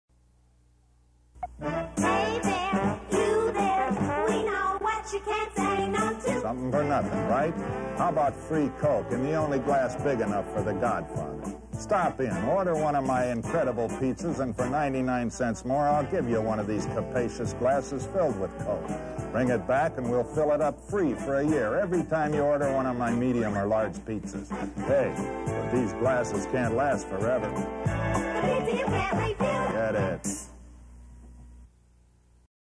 Big Enough Coke for the Godfather- Vintage Godfather's Pizza Commercial
Come in for a Coke in the only glass big enough for THE Godfather!